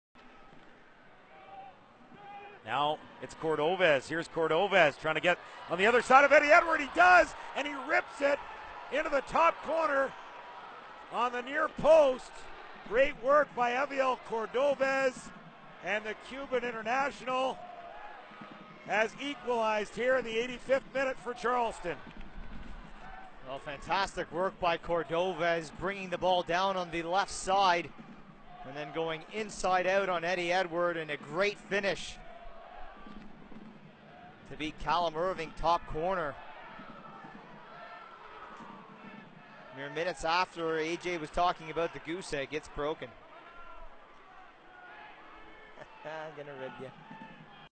Heviel Cordoves Wonder Goal v Ottawa
Golazo from Heviel Cordoves for Charleston Battery against Ottawa Fury in the USL